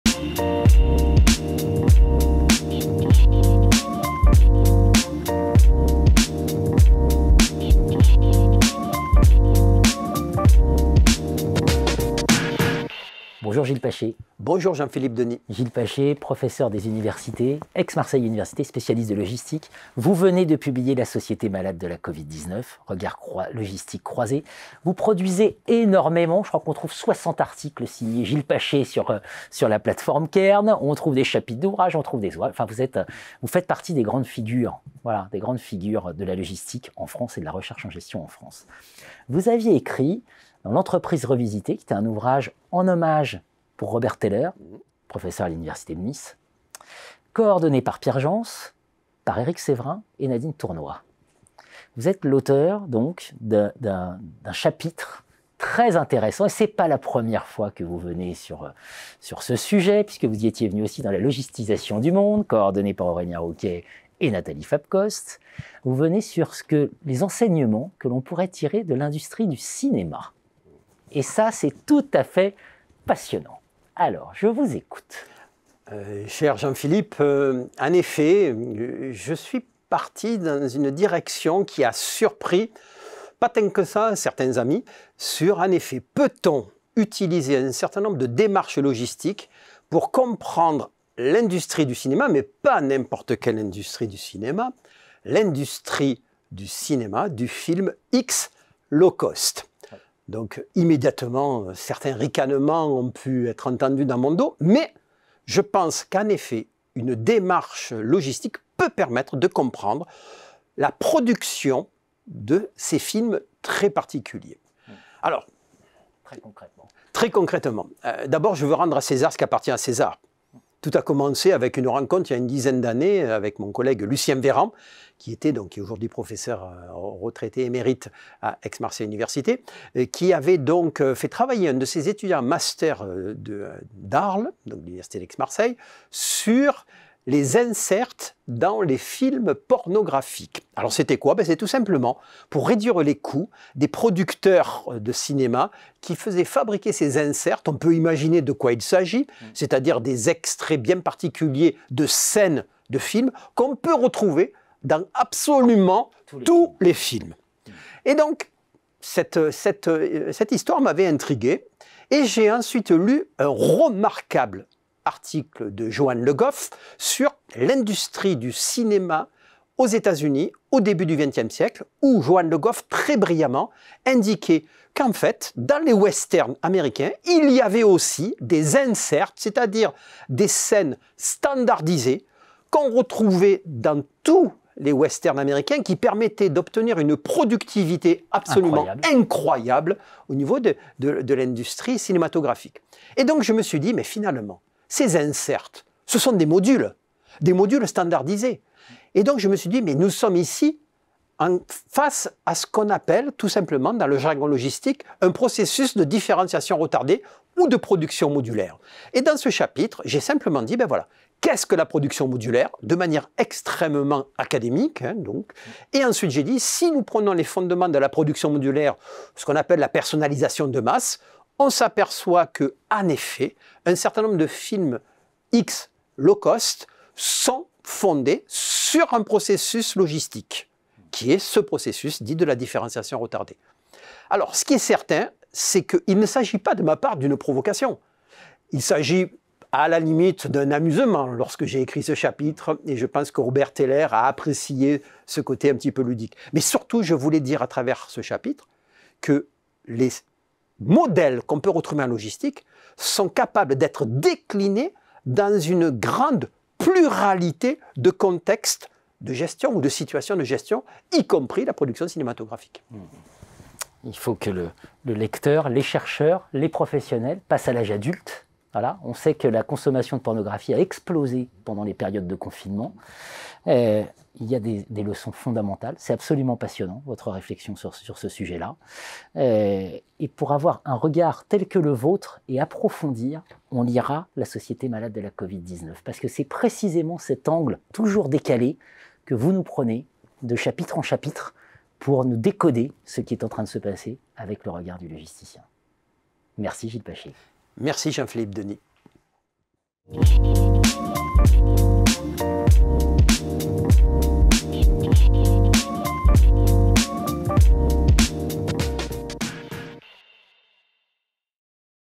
La logistique de l’industrie du film X [Gilles Paché]
Xerfi Canal a reçu Gilles Paché, professeur des Universités en Sciences de Gestion et du Management, AMU - Aix-Marseille Université Laboratoire : CERGAM (AMU - Aix-Marseille Université), pour parler de la logistique.  Une interview menée par Jean-Philippe Denis.